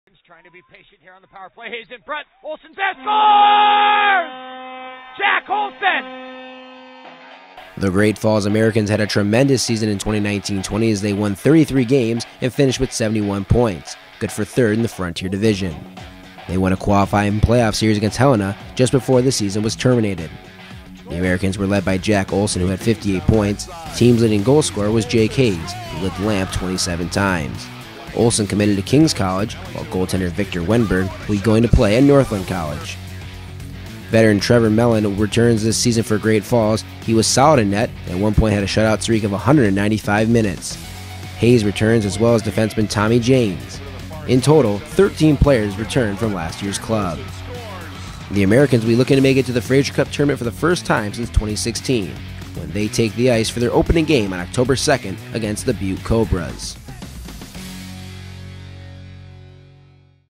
3View - Great Falls
The Great Falls Americans are gearing up for the 2020-21 season!